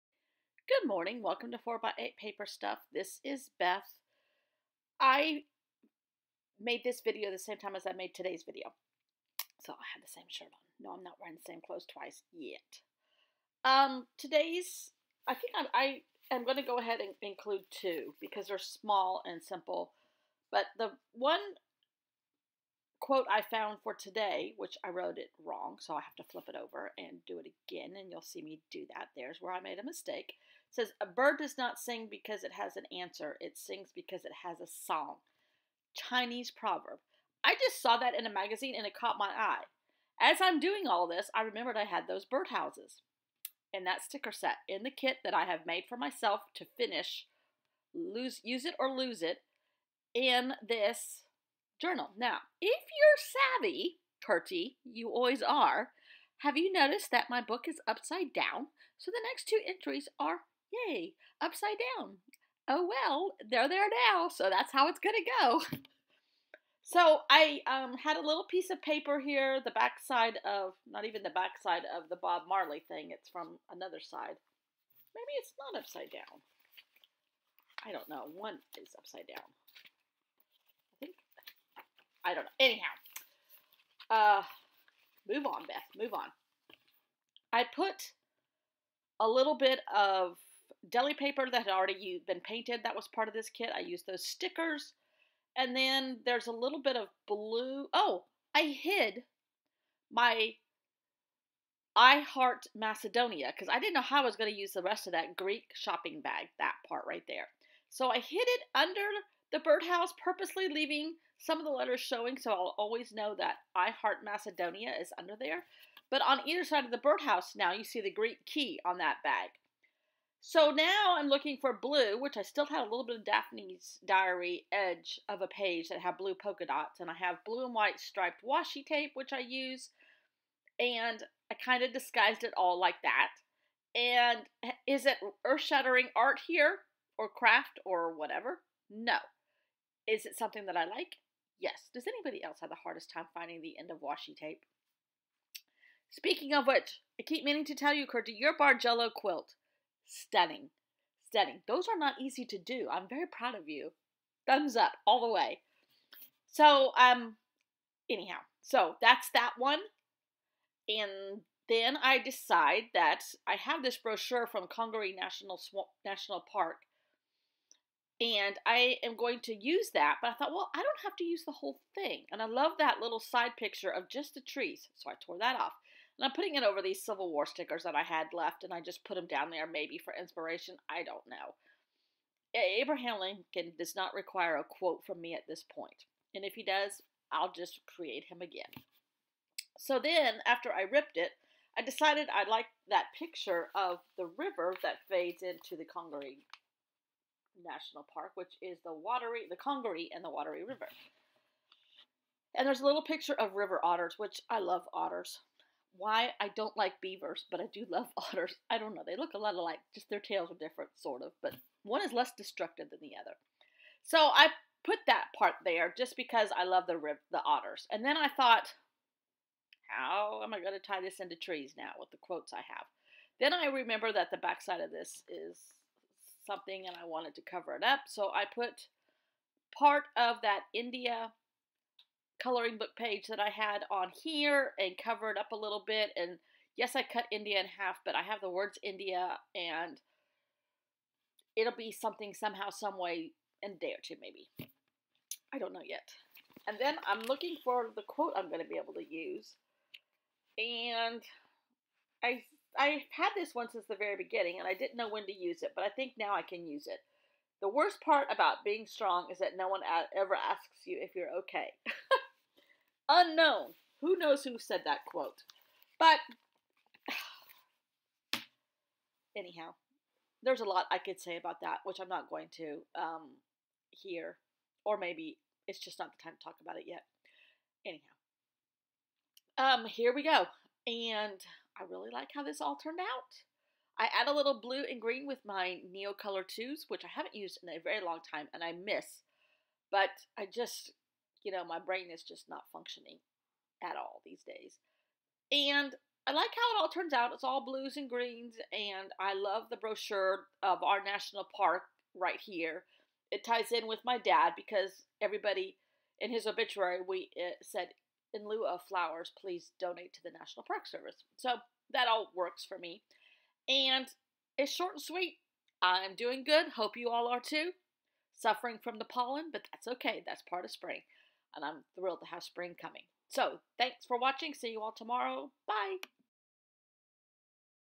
Junk Journal With Me, Ep16
I make two real quick spreads with 2 quotes.  I may or may not have done these spreads upside down??  Beth